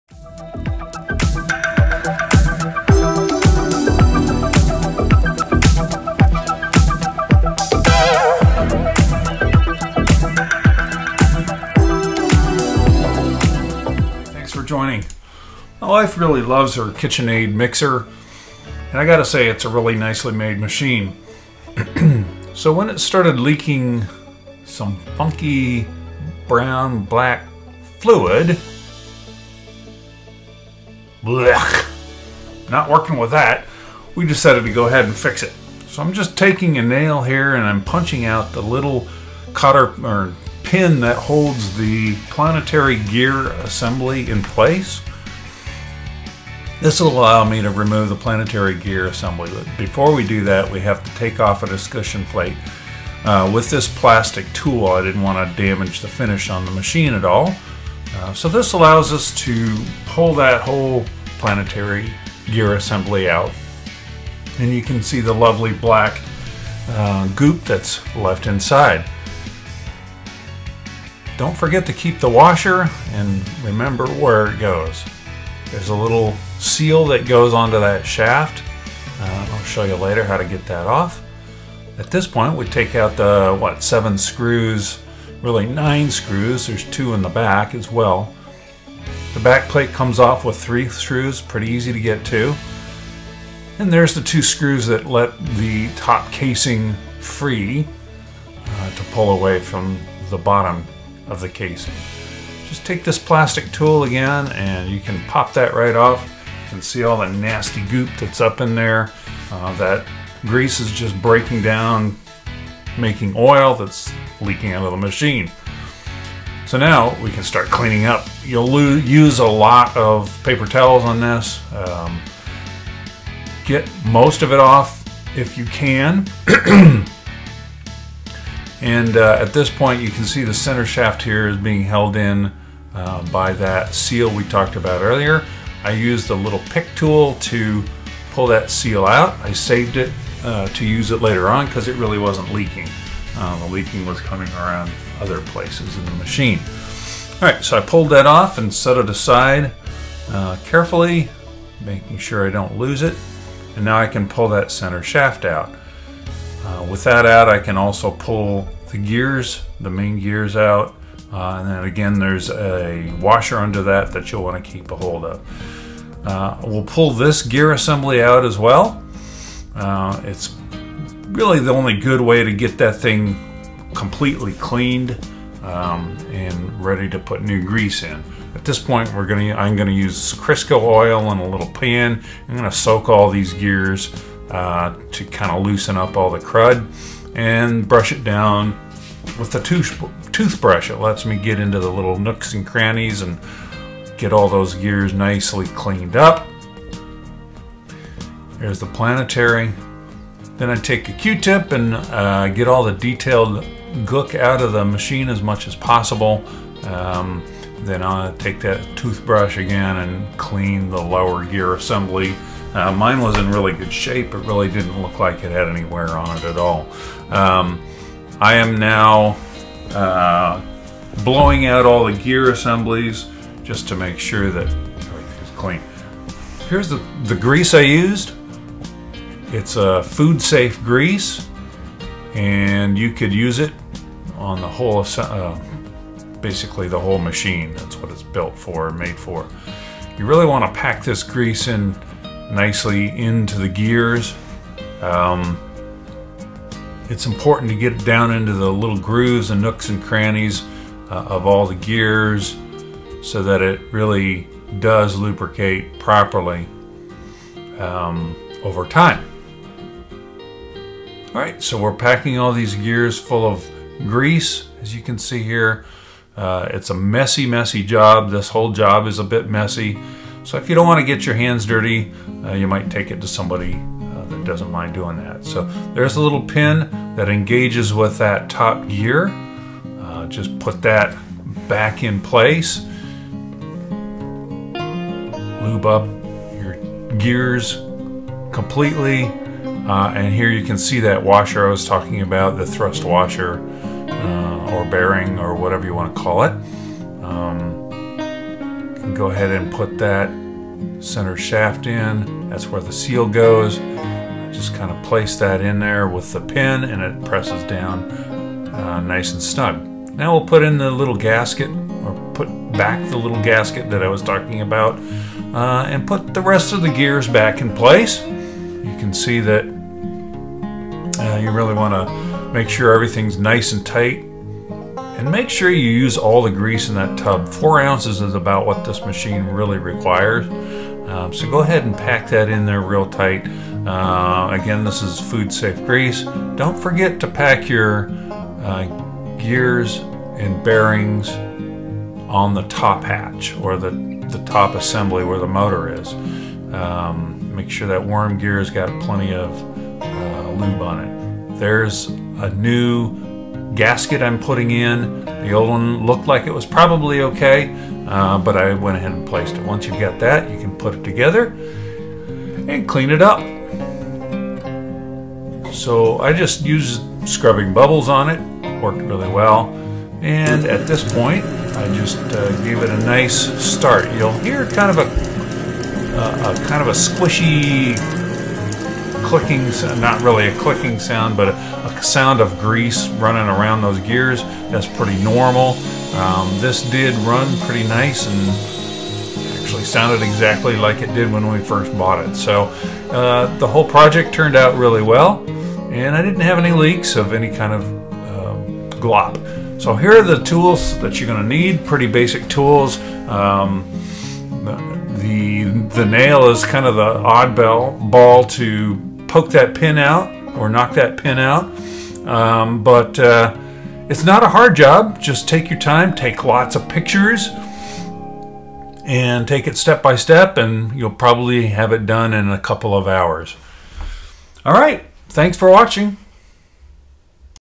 KitchenAid Artisan leaky oil fix
This will show how to fix a black oily leak these artisan mixers develop.
the root of the problem is the grease is breaking down turning to oil and leaking out.  this shows how to remove the motor assembly and gears in order to clean out all the old grease so you can repack the bearings and gears.